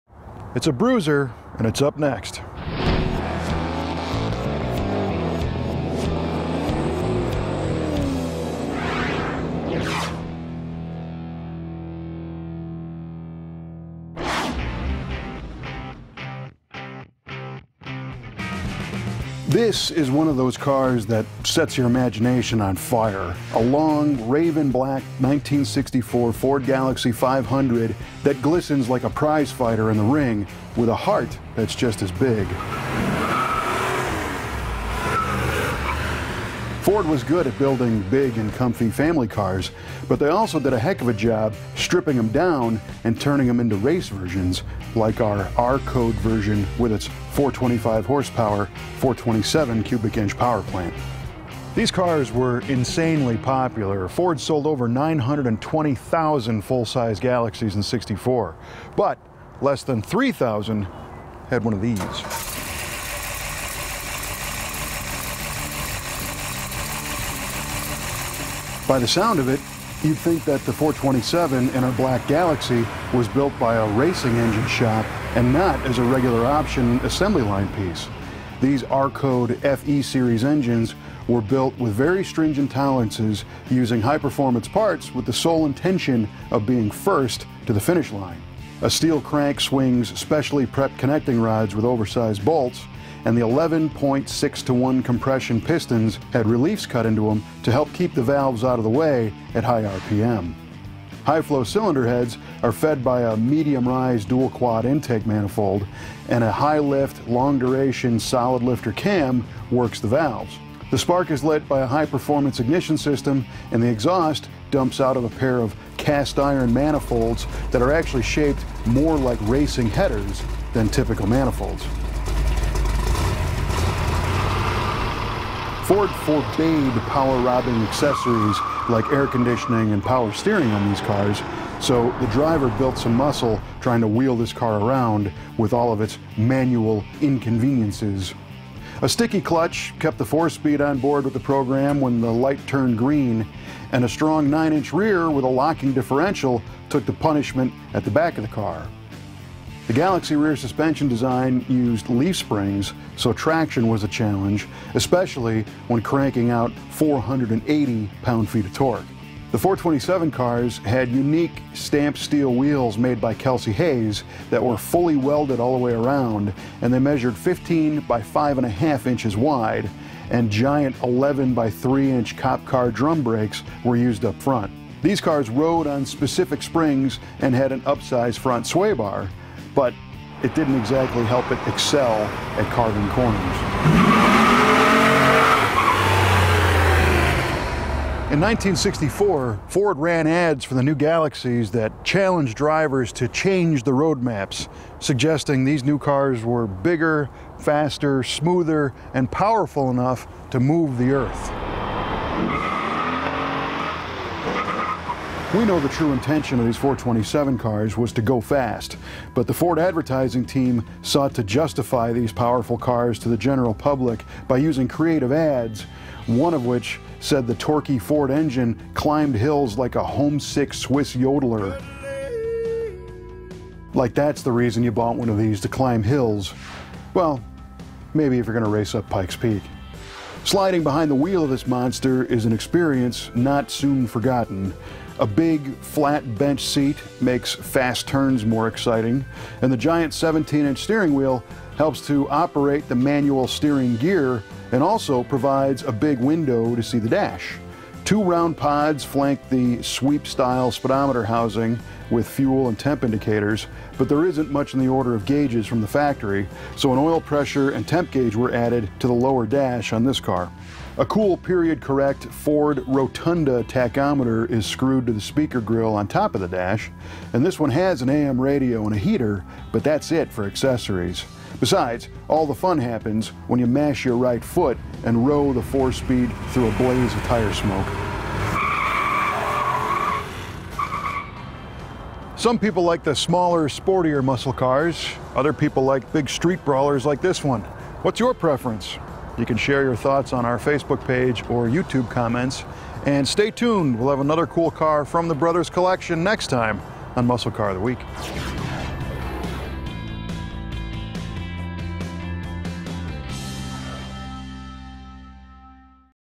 Muscle Car Of The Week Video Episode #190: 1964 Ford Galaxie 500 427 4-Speed R-Code V8TV
This is one of those cars that sets your imagination on fire.   A long, Raven black ‘64 Ford Galaxie 500 that glistens like a prize fighter in the ring, with a heart just as big.   Ford was good at building big and comfy family cars, but they also did a heck of a job stripping them down into racing versions, like our R-code version with it’s 425 HP 427 powerplant.  From The Brothers Collection.